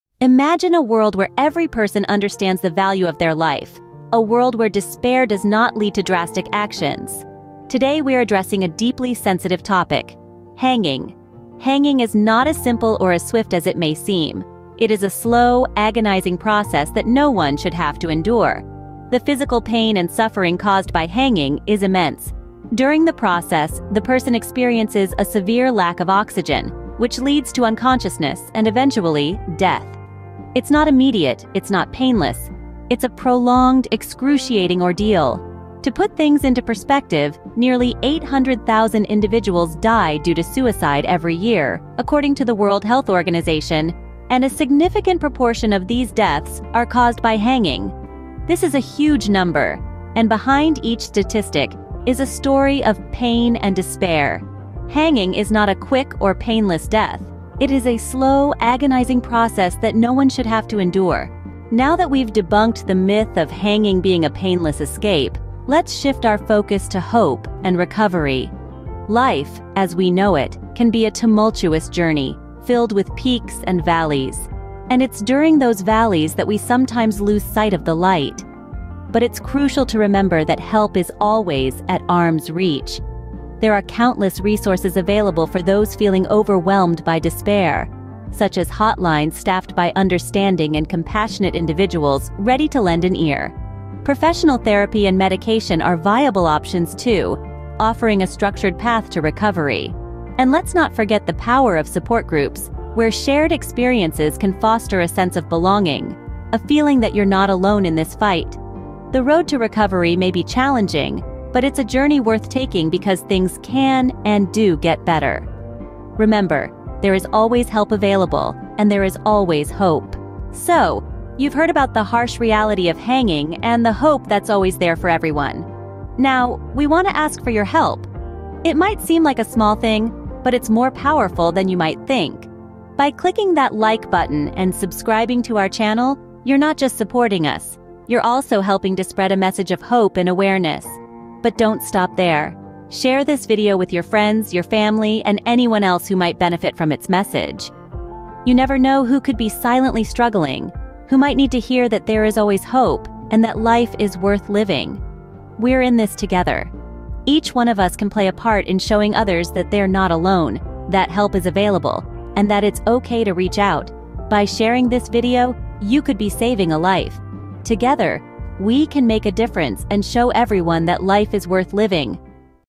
Have you Frustrated in life_Hope on living | Motivational video for suicide attempt
Content Warning: This video discusses the sensitive topic of suicide by hanging.

In this powerful video, we delve into the harsh realities of death by hanging. Through a thoughtful and compassionate lens, we aim to discourage this tragic act while promoting crucial mental health awareness.

Suicide is a complex issue, often rooted in unimaginable pain and struggle. By understanding the gravity of this topic, we hope to inspire empathy, provide resources, and encourage those in need to seek help.

Remember, you are not alone. There are always alternatives, and your life has immeasurable value. If you or someone you know is experiencing a mental health crisis, please don't hesitate to reach out for support.

Together, let's break the silence, end the stigma, and work towards a world where everyone feels heard, valued, and empowered to choose life.